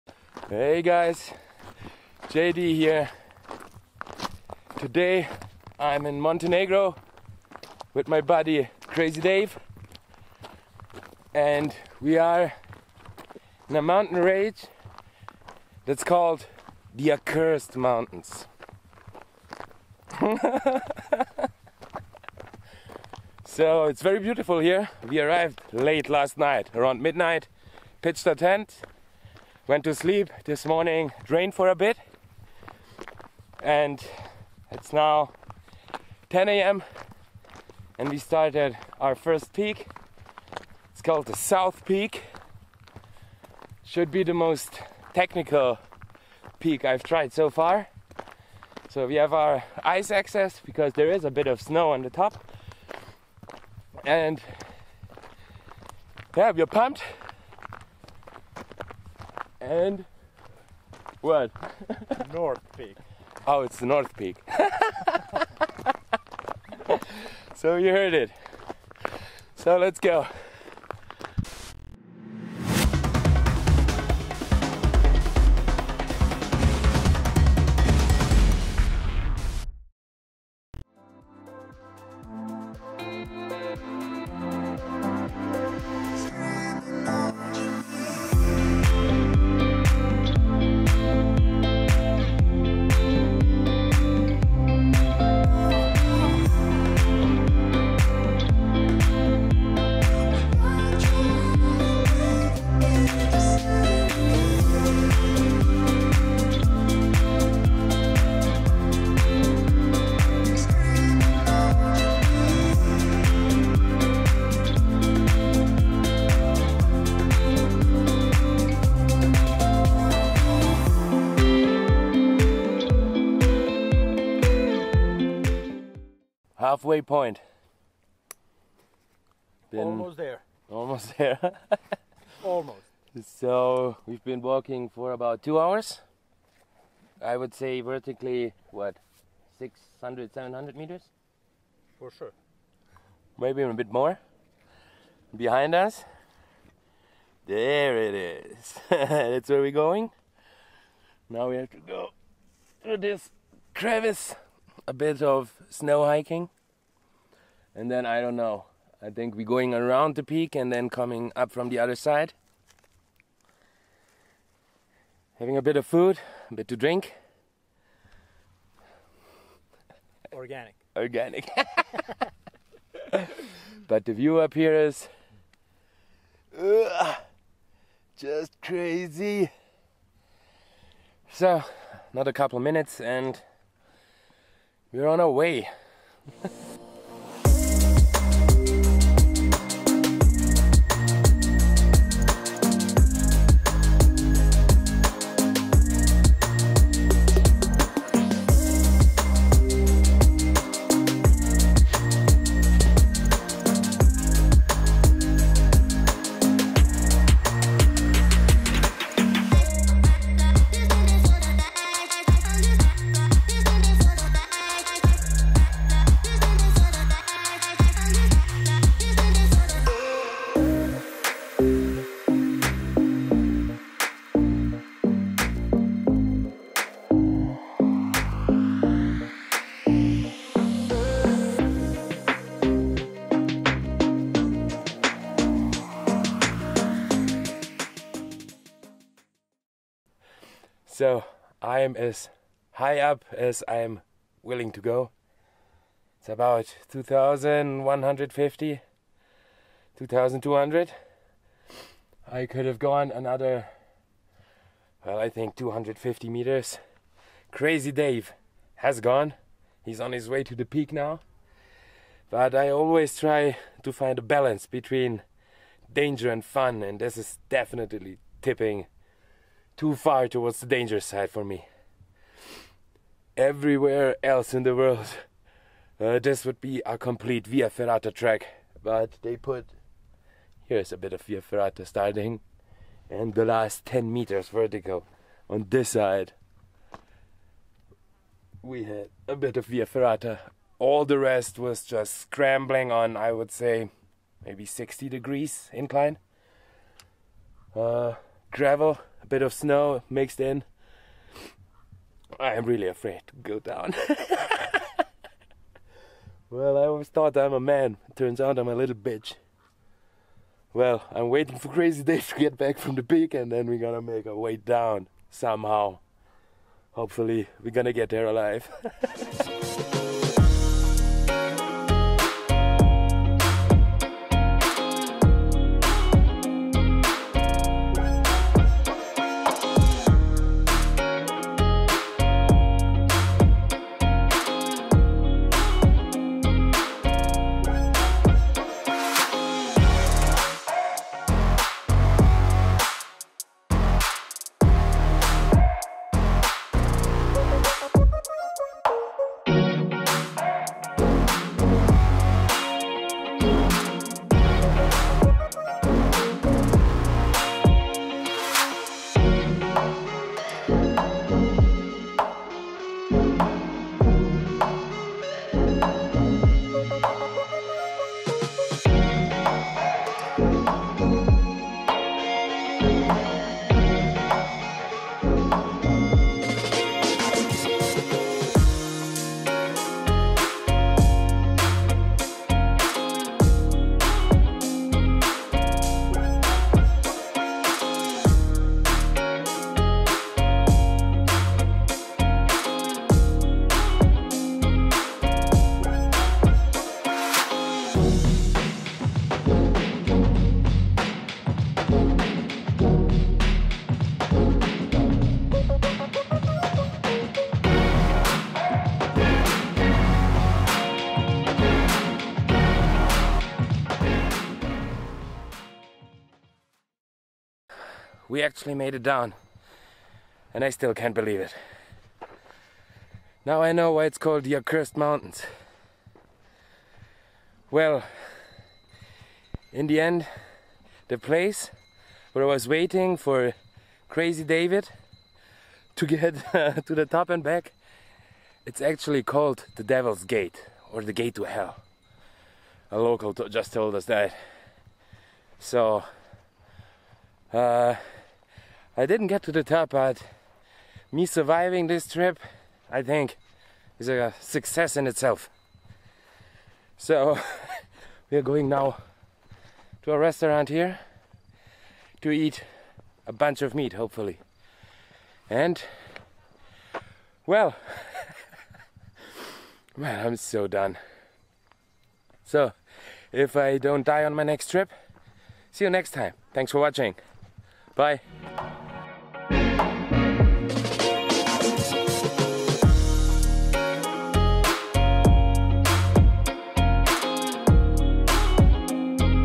Accursed Mountains - North Peak climb (fail)
The Accursed mountains are a mountain group in the western part of the Balkans, and are part of the Dinaric Alps range.
The climb is very steep with several (YDS) class 3 rock scrambling sections.
The nature is amazing, and if you're experienced enough, North peak shouldn't be a problem for you.

Cheers.